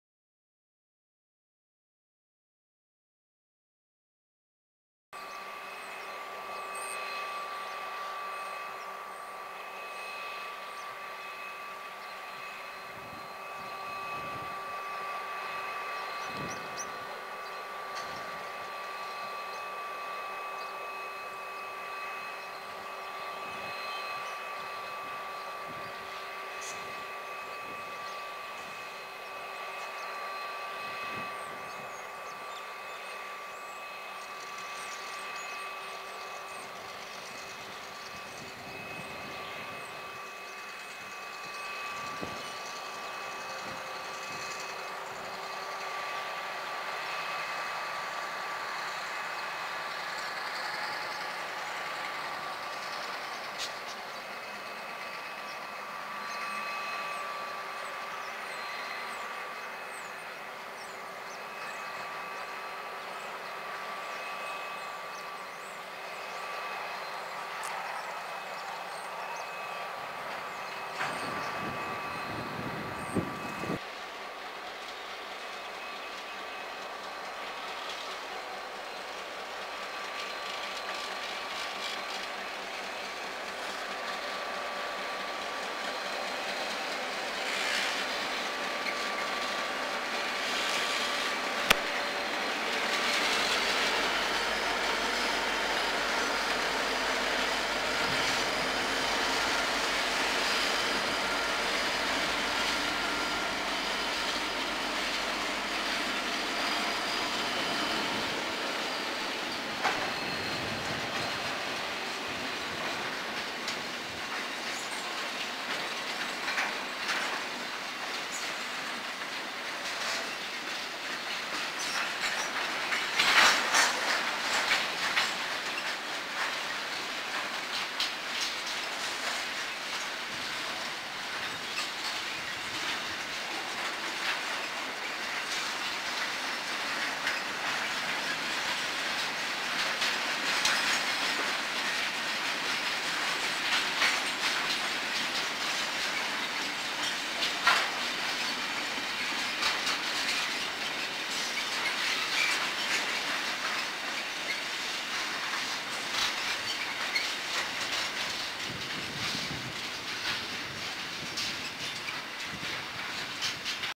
GM36-G516 CRT shunt Tottenham Loop 2nd August 2005
GM36 used to lead these shunts every so often.